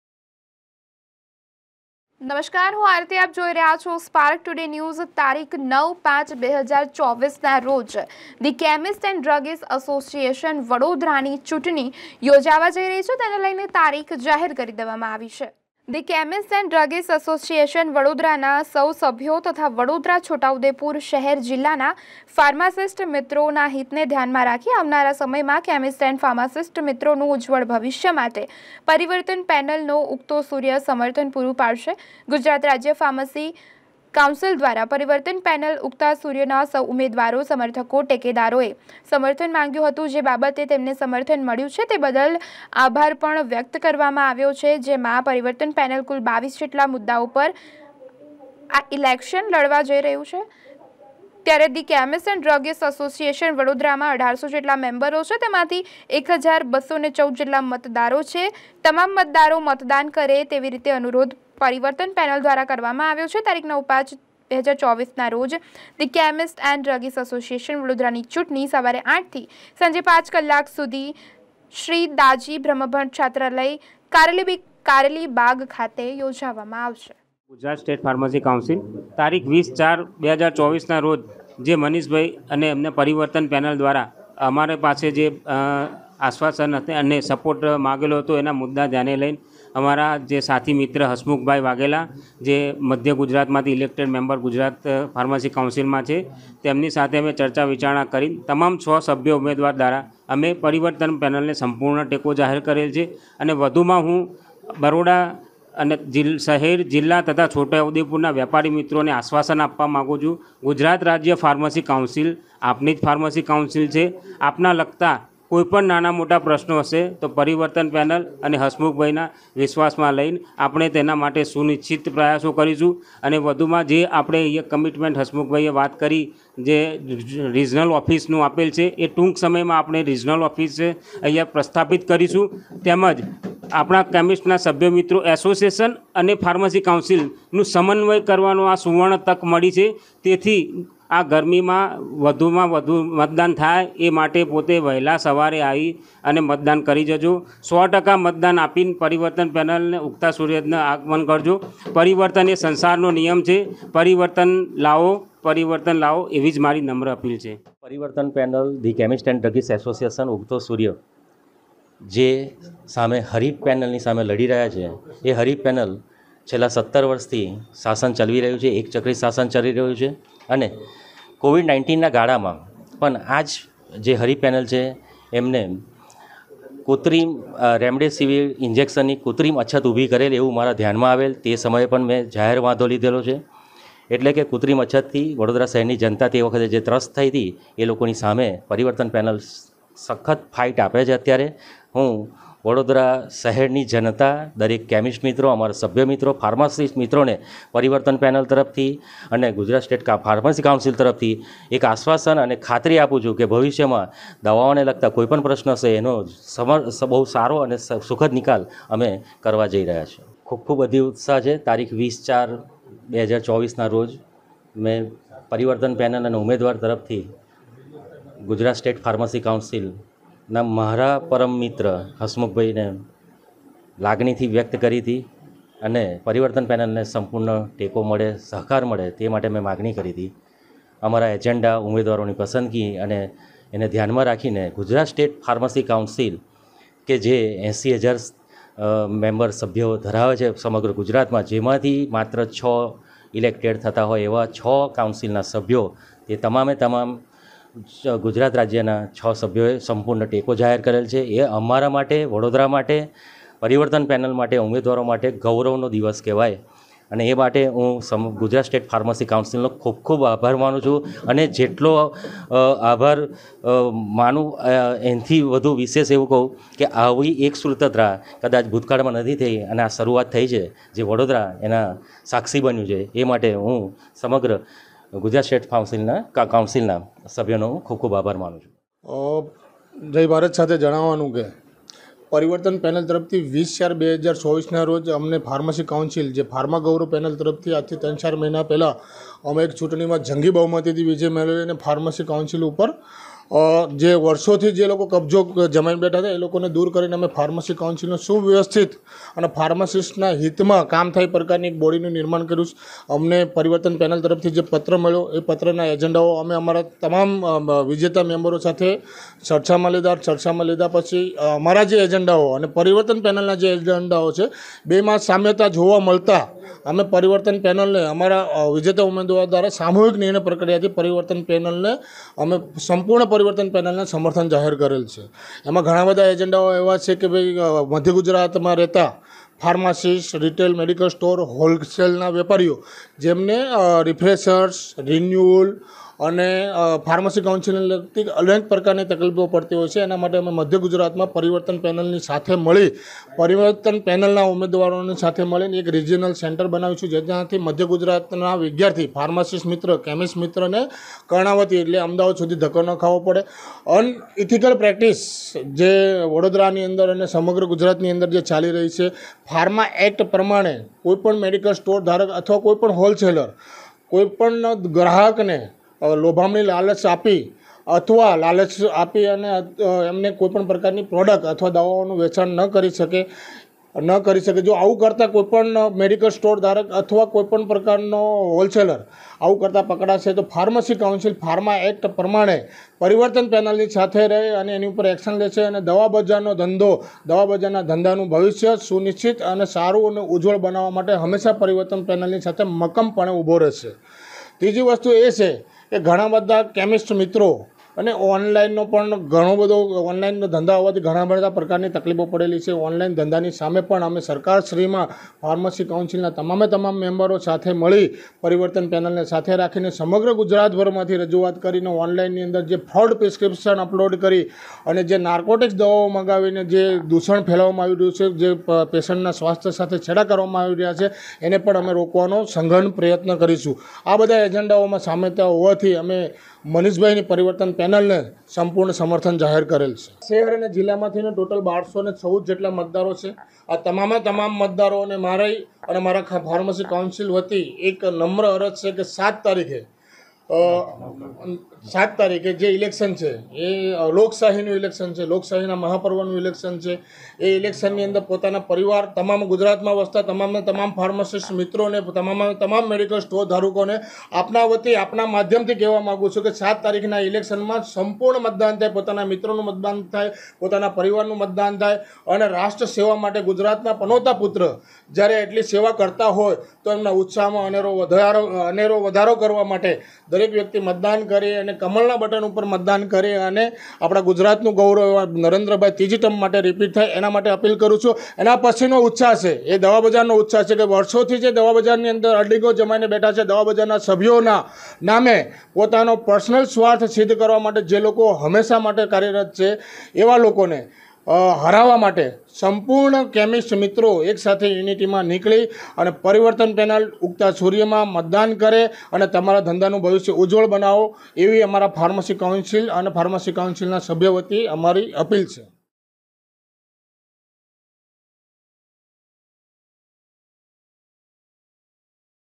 ધી કેમેસ્ટ એન્ડ ડ્રગિસ્ટ એસોસિએશન વડોદરાની ચૂંટણી યોજાશે તારીખ થઈ જાહેર 2024 | Spark Today News
ધી કેમેસ્ટ એન્ડ ડ્રગિસ્ટ એસોસિએશન વડોદરાની ચૂંટણી યોજાશે તારીખ થઈ જાહેર